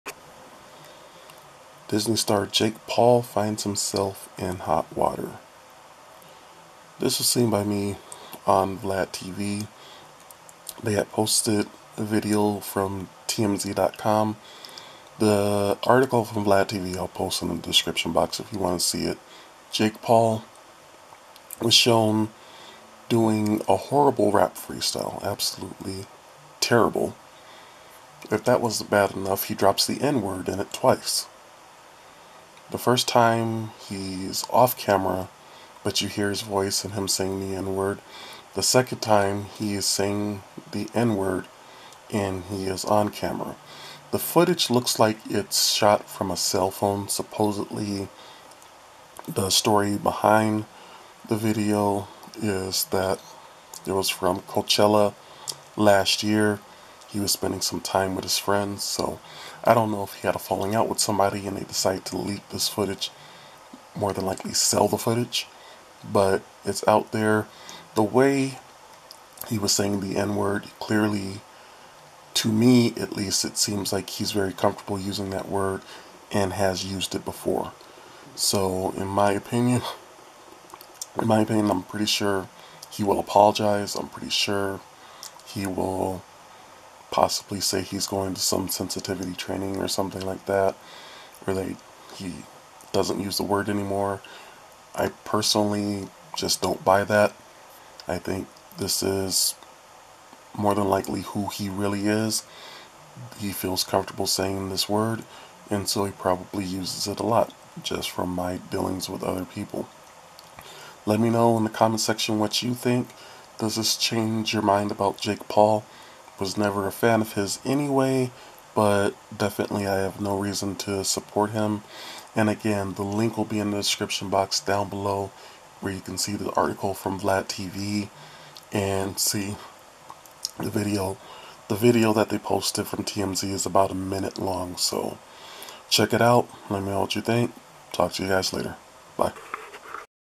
Jake Paul Says N-Word In Horrible Freestyle
Jake Paul from Disney fame is in trouble for saying the n-word in a freestyle that was supposedly shot on a phone last year.

Blog post can be read at waygookinway.com